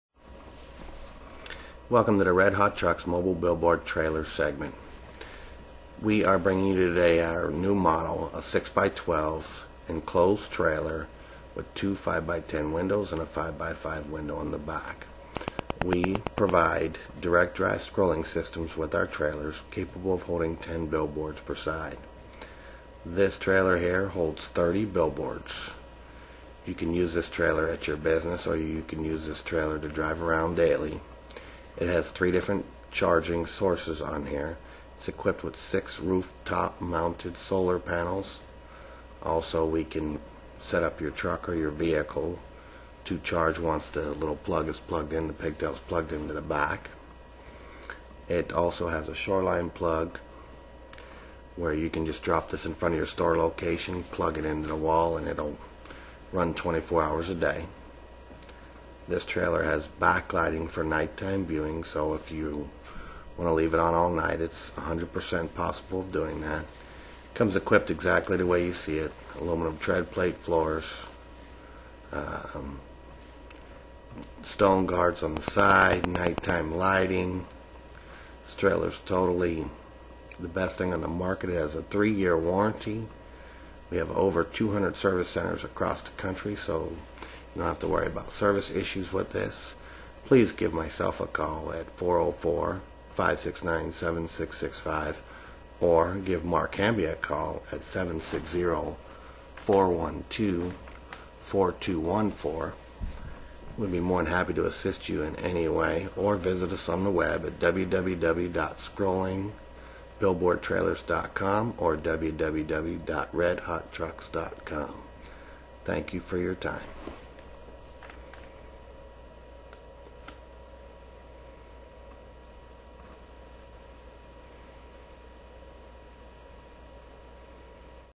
Scrolling Billboard Trailers
Scrolling Billboard Trailers are the new tool for business owners to deliver their message to the customers they want, where those customers make a tremendous number of their buying decisions; IN THEIR CARS.

Drive your billboard trailer to the right locations at the right time and watch your business increase.